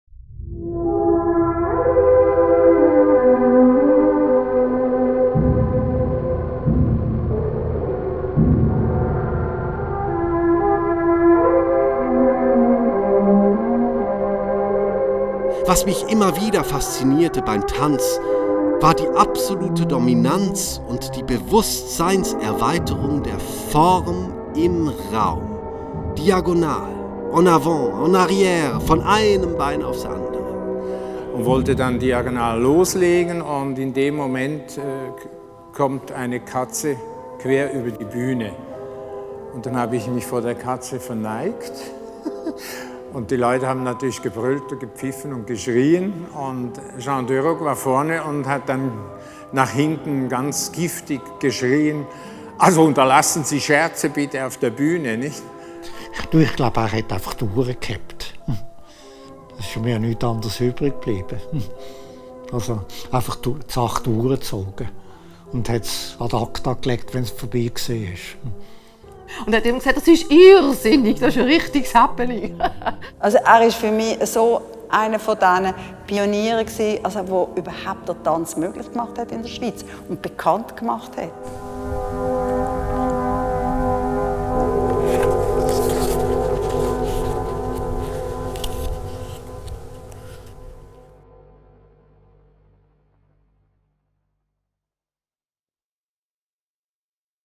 Eine Tanzstunde mit Jean Deroc (TRAILER) - Fabian Chiquet / Kunsthaus Zofingen)
Ausstellung vom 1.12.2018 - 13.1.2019 im Kuntshaus Zofingen.
Live Performances mit Bruno Spörri am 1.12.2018 & 5.1.2019.

EINE TANZSTUNDE MIT JEAN DEROC
Eine filmisch Installation von Fabian Chiquet.

Eine Tanzstunde mit Jean Deroc ist eine Interpretation zur Arbeit des schweizer Choreografen Jean Deroc (*1925 – † 2015) aus der heutigen Sicht des jungen Pop-, Theater- und Videokünstlers Fabian Chiquet. Die Inszenierungen im Kunsthaus Zofingen sind keine linearen Geschichtsstunden oder Präsentationen von Derocs choreographischer Arbeit, sondern sie tragen vielmer seinem Wesen mit den pathetischen, wie auch gewitzten Zügen Rechnung und zeigen, weshalb er für die heutige Tanzszene wichtig war. Entstanden ist ein Hörspiel, das man zwei mal live erleben kann, sowie eine Installation aus mehreren Videoportraits mit Wegbegleitern Deroc's, die aufzeigen was für ein einnehmender und begeisterungsfähiger Mensch er war.

Videoportraits:
Armin Wild / Christina Meyer / Daniell Ficcola / Georg Weber / Monika Linder / Markus Bühlmann / Evi Birmele / Bruno Spörri